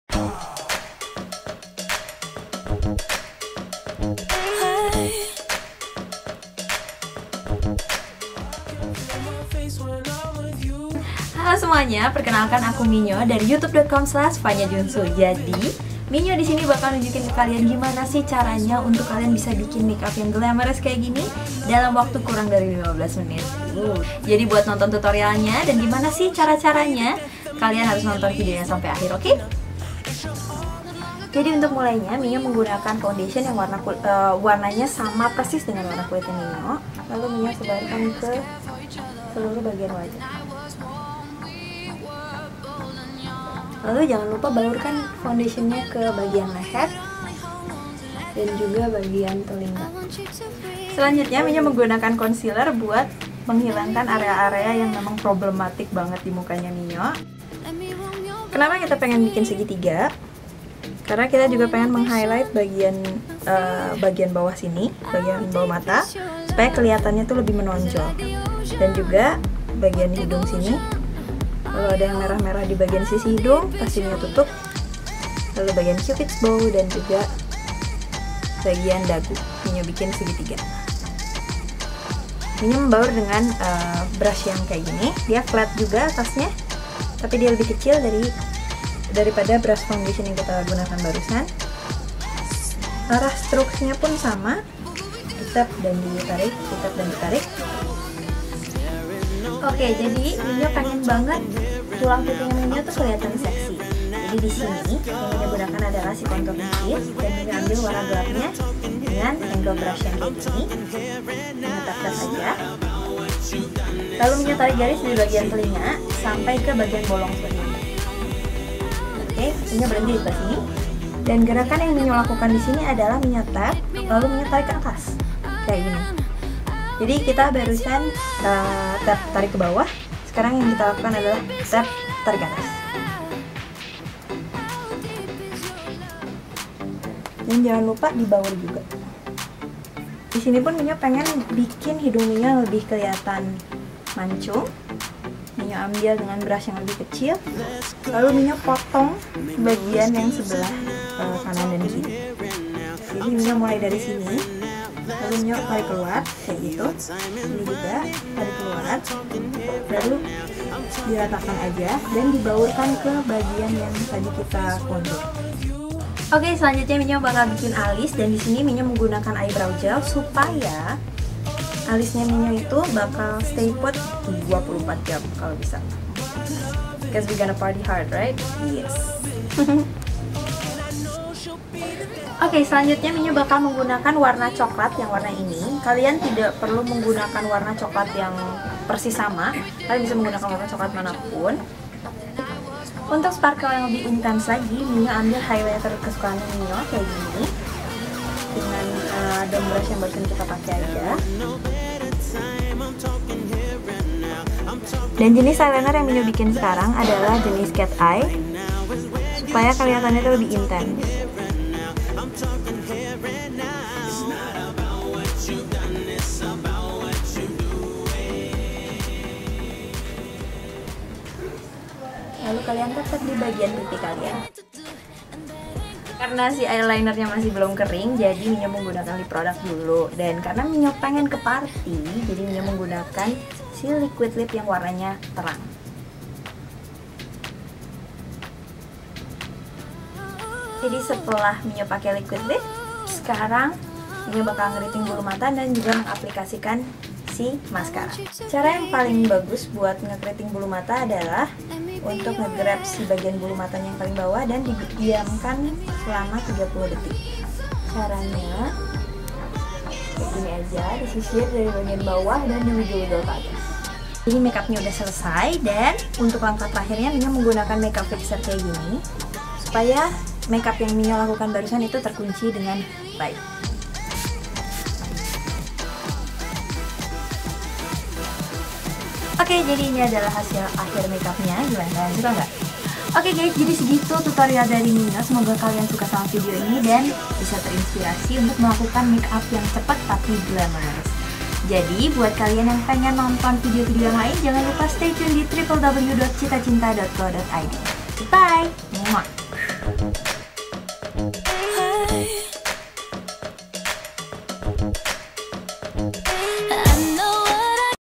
Tip Makeup ke Pesta ala Minyo (Before-After)
Tertarik belajar makeup sendiri untuk ke pesta? Ada tip dari Vlogger Minyo33 yang bisa dipraktekkan. Caranya mudah dan cepat, kok. Seperti apa video tutorialnya? Intip video berikut ini ya

Ingin melihat video tutorial dari Minyo33 yang lainnya bisa lihat juga di